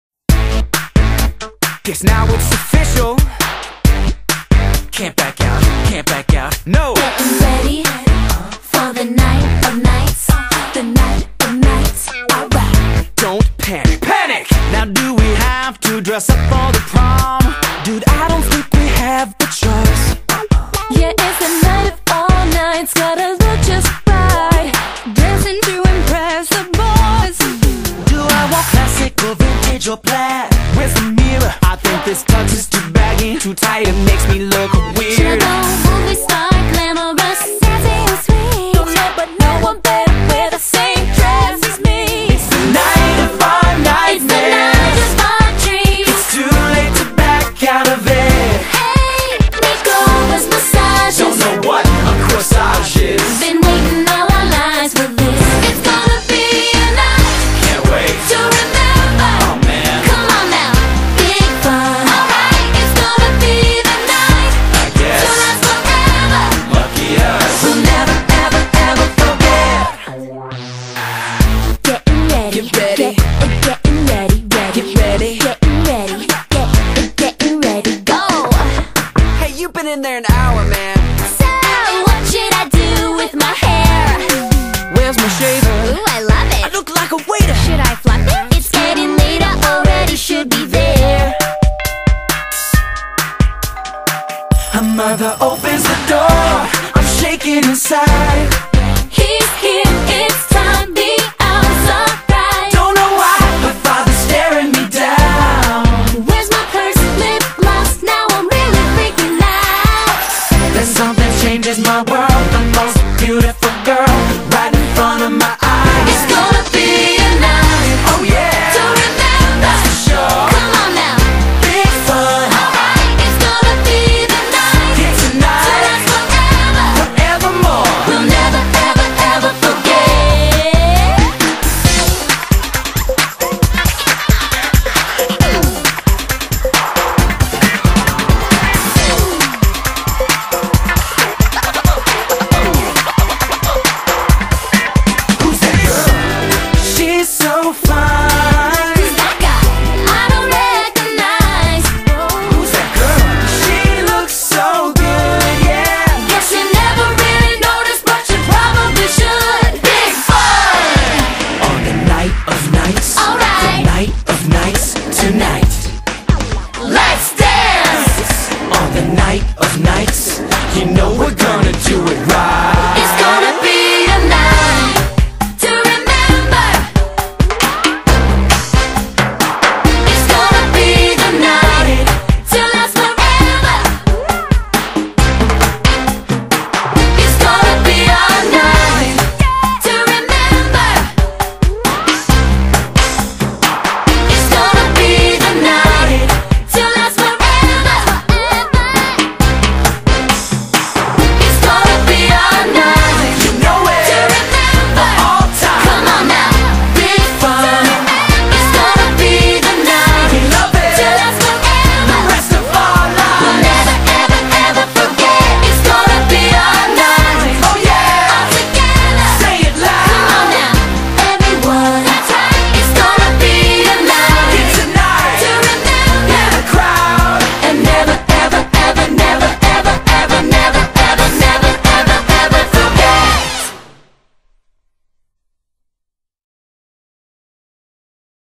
Fareez Cullen and Esha Birthday Party (Prom Night 2009)
Slide photos of my birthday party.
Theme : Prom Night 2009